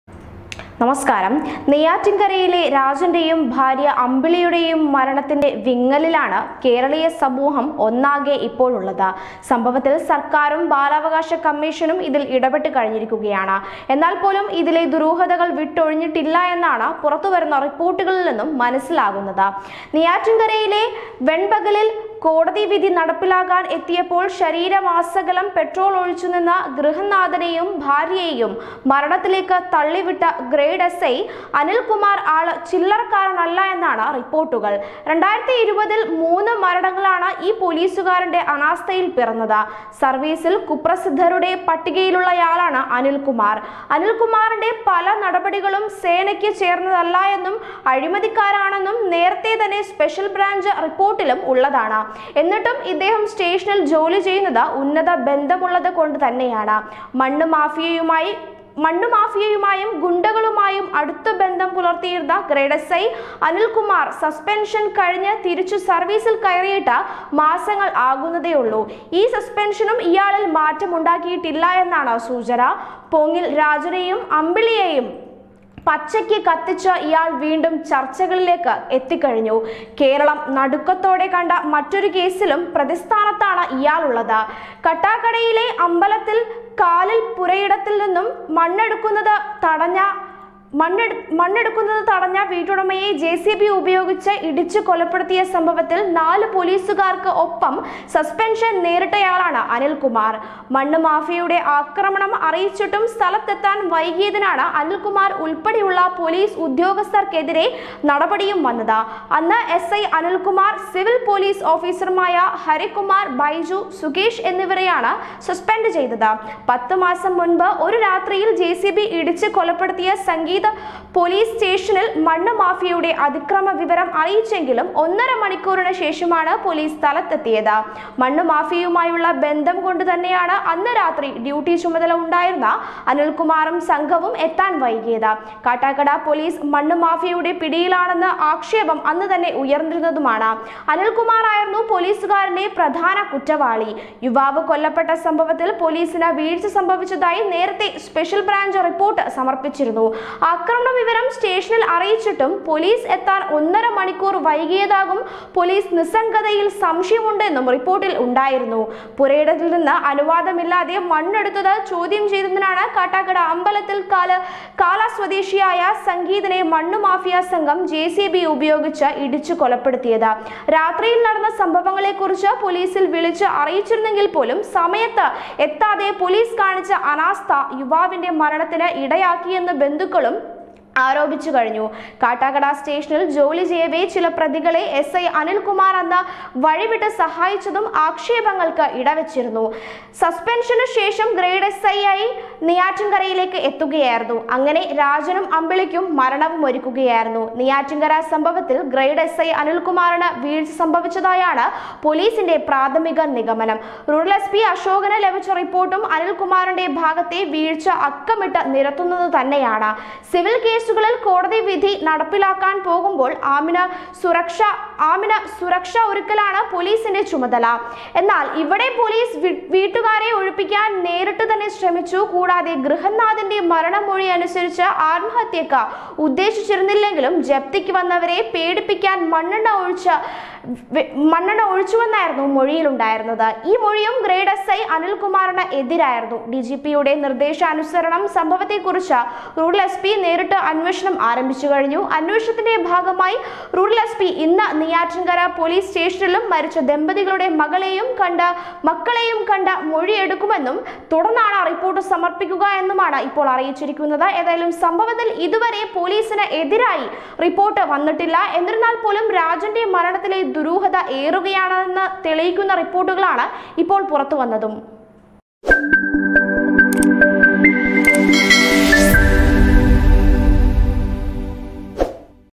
നെയ്യാറ്റിന്‍കരയിലെ ക്രൂരന്‍ എസ് ഐ അനില്‍കുമാര്‍ സേനയിലെ സ്ഥിരം വില്ലന്‍|NEYYANTTINKARA SI ANILKUMAR
നെയ്യാറ്റിന്‍കരയിലെ രാജന്റെയും ഭാര്യ അമ്പിളിയുടെയും മരണത്തിന്റെ വിങ്ങലിലാണ് കേരളീയ സമൂഹം ഒന്നാകെയുള്ളത്.സംഭവത്തില്‍ സര്‍ക്കാരും ബാലാവകാശകമ്മീഷനും ഇതില്‍ ഇടപ്പെട്ടു കഴിഞ്ഞു.എന്നാല്‍പോലും ഇതിലെ ദുരൂഹതകള്‍ വിട്ടൊഴിഞ്ഞിട്ടില്ലെന്നാണ് പുറത്ത് വരുന്ന റിപ്പോര്‍ട്ടുകളില്‍ നിന്നും മനസ്സിലാവുന്നത്.